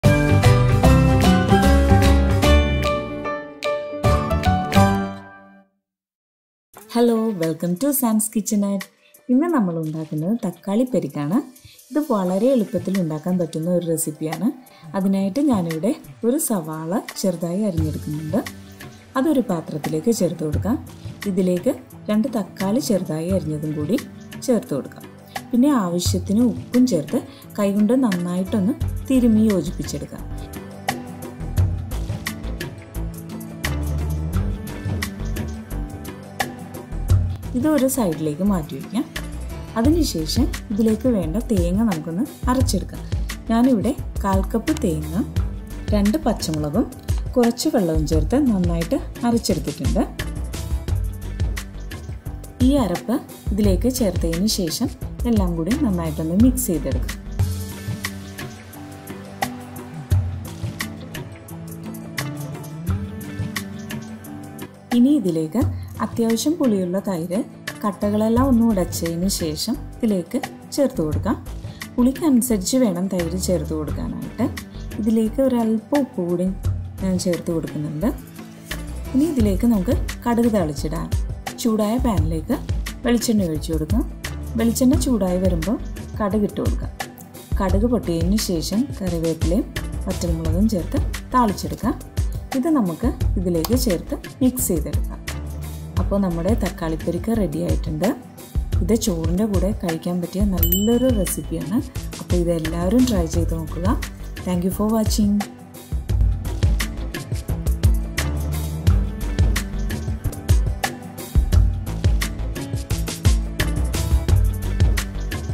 തക്കാളിപെരുക്ക് | thakkali perakku | ഞൊടിയിടയില്‍ ഉണ്ടാക്കാന്‍ പറ്റിയ ഒരു സിമ്പിള്‍ കറി| Vlog-03
Thakkali perukku , sans kitchenette
Ingredients

Tomato -2
Onion -1
Yoghurt -1/2cup
Salt
Dried red chili -2
Curry leaf
Water

Sans kitchenette
thakkali perakku,